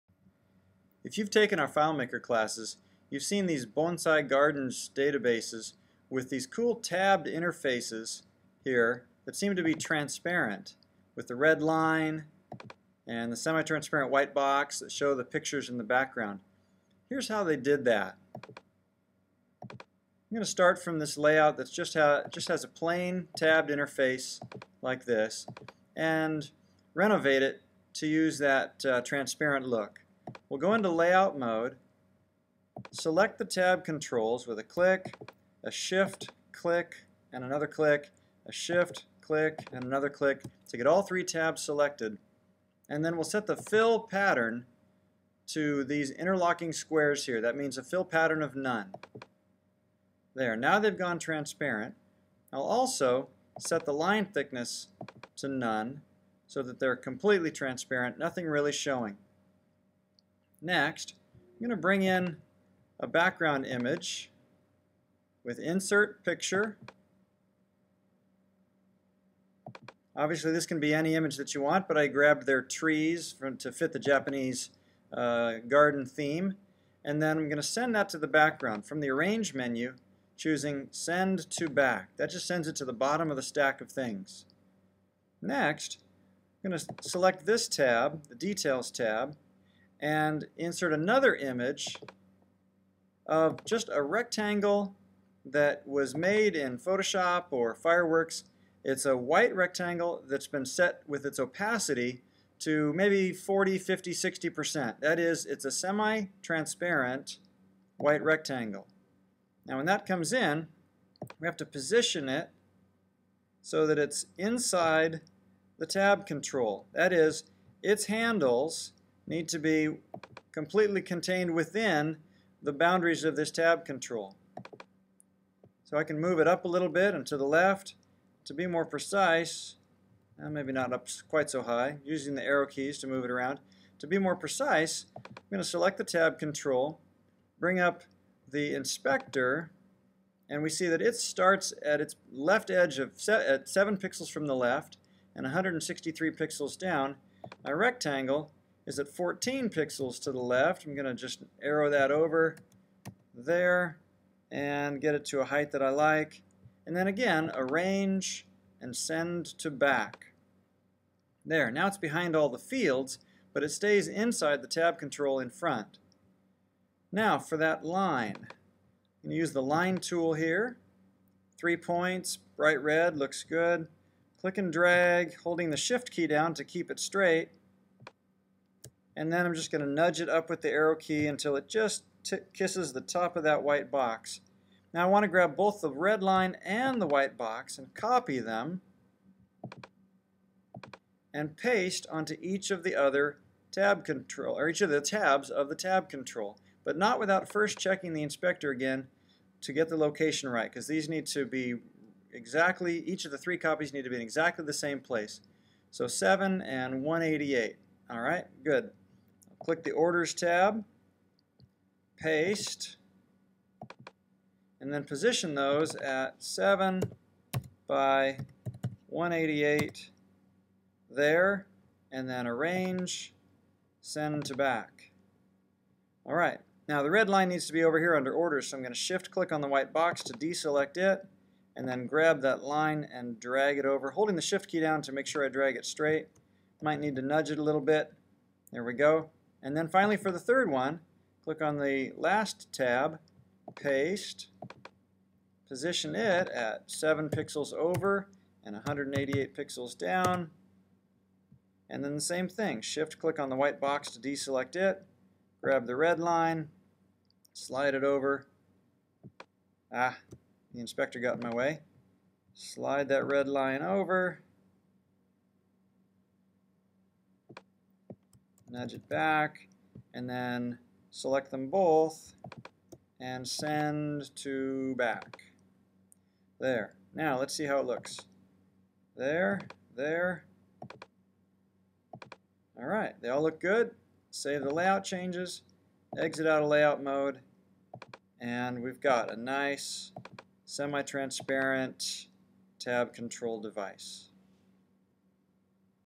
FileMaker Pro 11 Training: How to create transparent Tab Controls
This video shows you how to redesign an ordinary Tab Control on a FileMaker Pro 11 database layout applying an elegant transparent design like that used in the FileMaker Training Series manual exercise files.